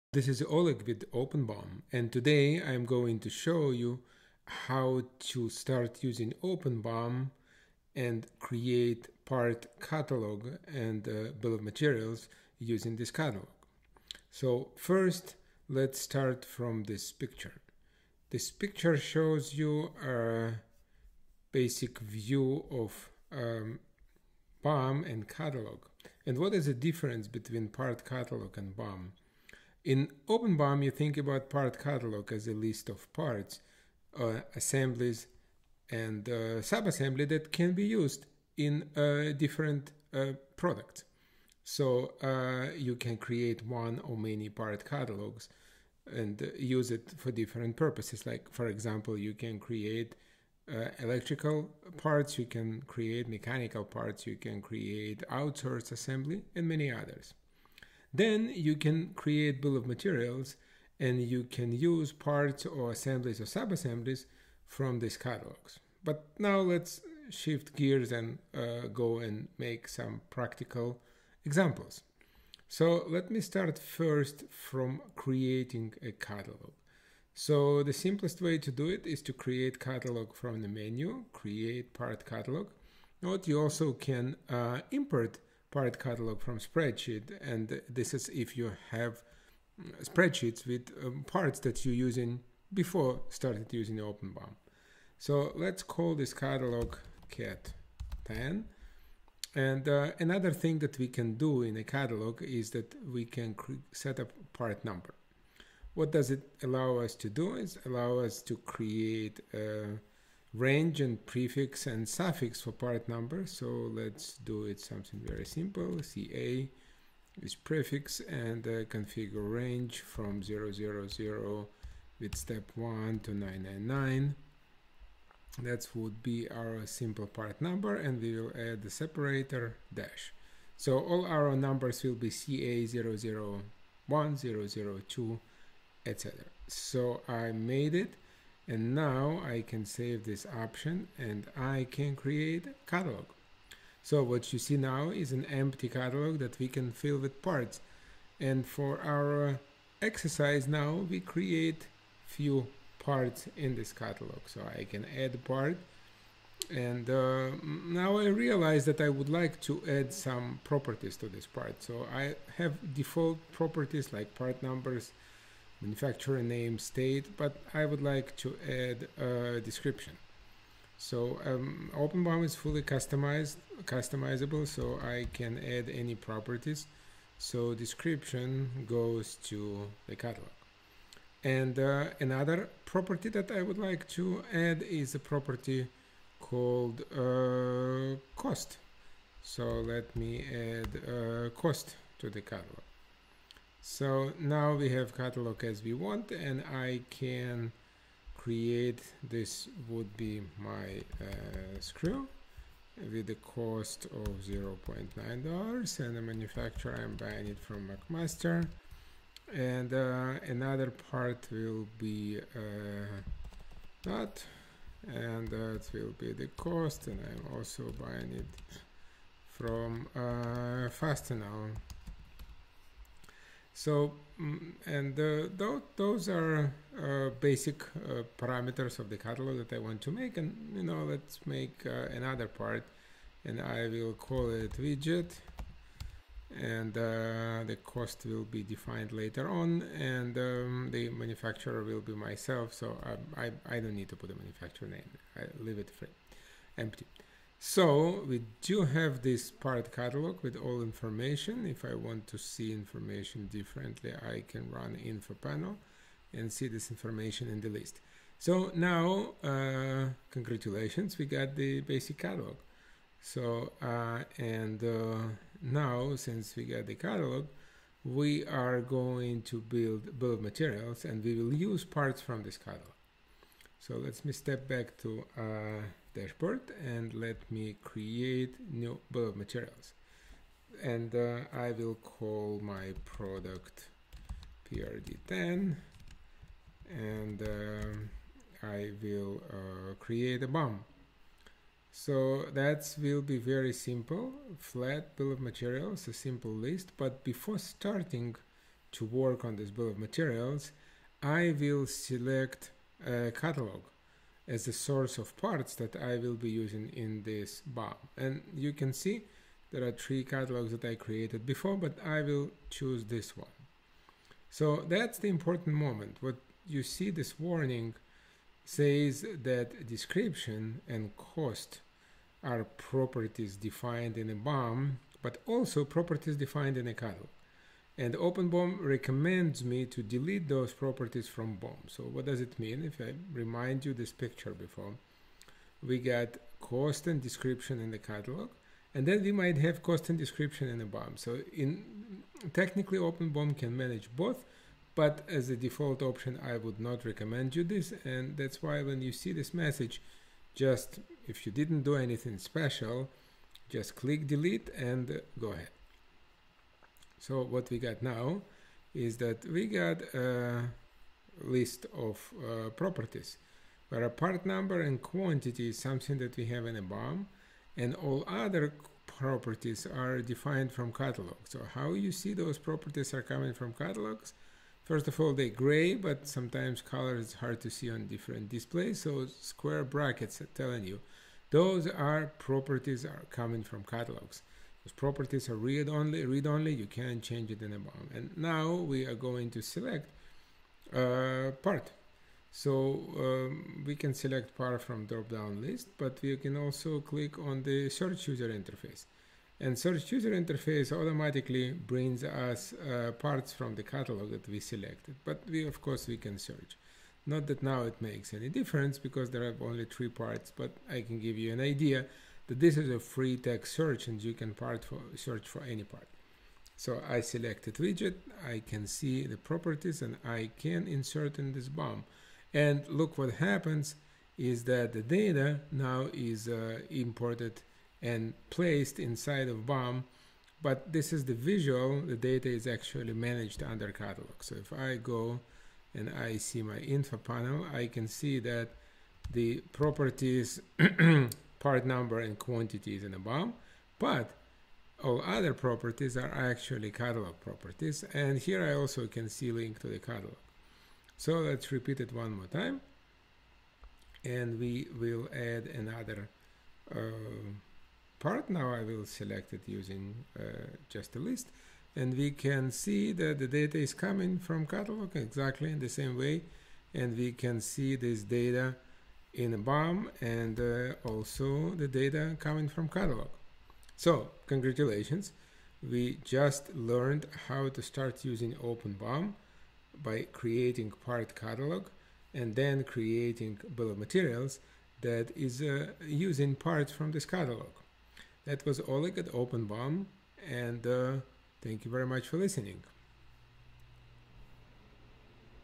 OpenBOM: step by step lesson how to create first part catalog and BOM
This video shows step-by-step instruction how to start using OpenBOM and create Part catalog and Bill of Materials